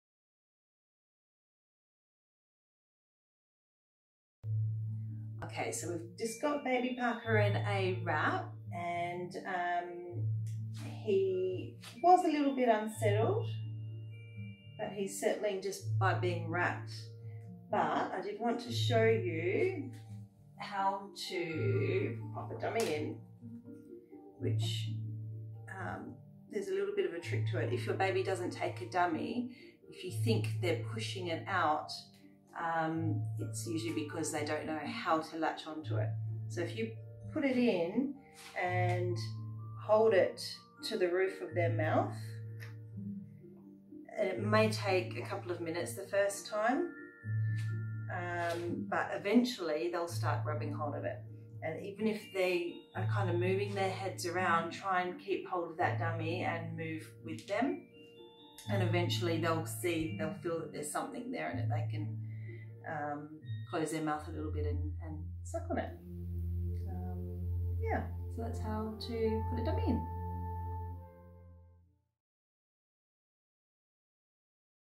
Newborn baby Dummy Tips
Hi Everyone, a lot of new parents tell me their baby doesn't like the dummy. In a lot of cases it's more that they don't know what they're suppose to do, they move their tongue around and it seems they push the dummy out.  This video shows the best way to help them to latch on to the dummy.